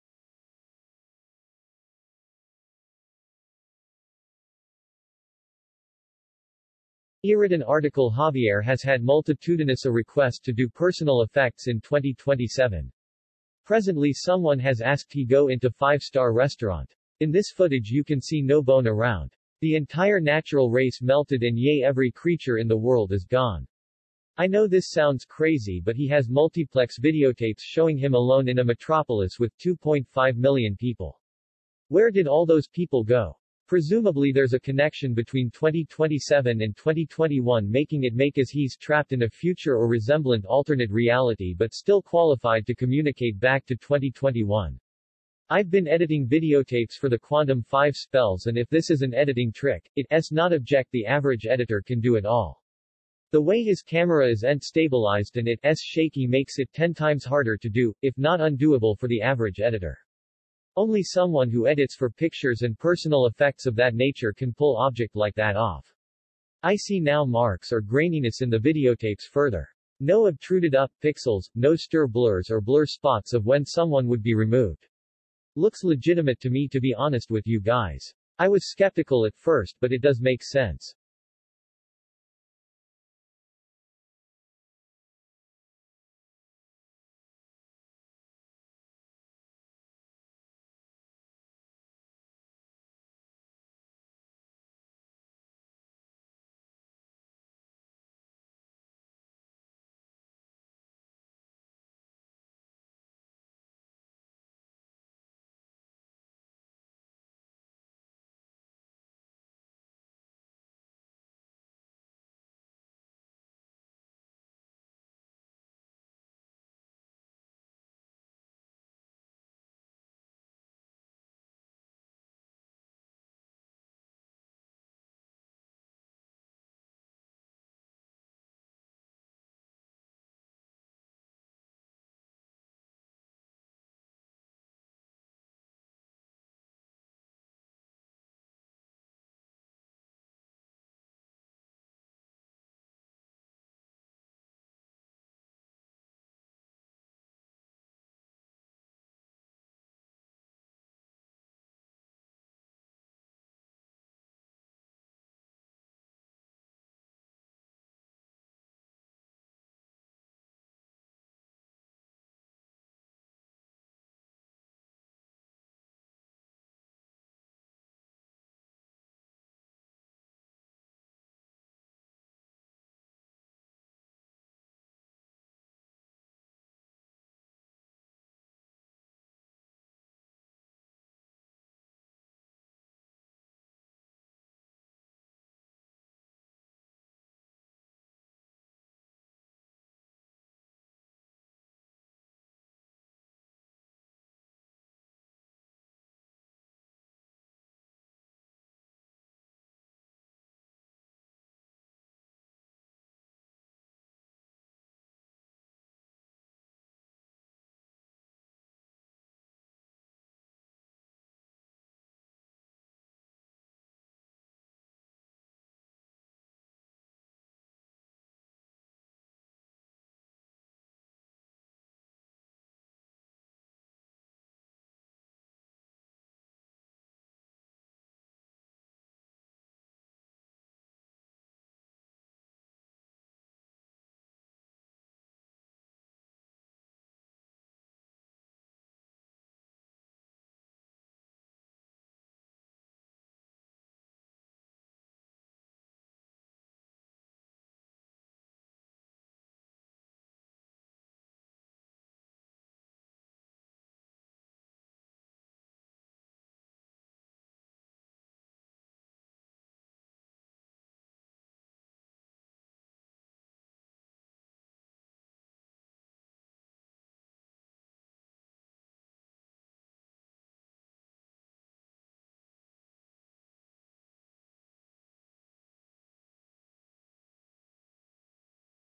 Time Travel man in 2027 ( JAVIER walks in Tunnel )
Javier has had multitudinous a request to do personal effects in 2027. Presently someone has asked he go into Five Star Restaurant. In this footage you can see no bone around.

The entire natural race melted and yea every creature in the world is GONE. I know this sounds crazy but he has multiplex videotapes showing him alone in a metropolis with2.5 million people. Where did all those people go? Presumably there's a connection between 2027 and 2021 making it make as he's trapped in a future or resemblant alternate reality but still qualified to communicate back to 2021.

I've been editing videotapes for the quondam 5 spells and if this is an editing trick, it ’s not object the average editor can do at all. The way his camera is n’t stabilized and it ’s shaky makes it 10 times harder to do, if not undoable for the average editor. Only someone who edits for pictures and personal effects of that nature can pull object like that off. I see now marks or grainyness in the videotapes further. No obtruded up pixels, no stir blurs or blur spots of when someone would be removed. Looks legitimate to me to be honest with you guys. I was skeptical at first but it does make sense.

Thanks everyone for there support

Subpals is great way to get more Subscribers to your channel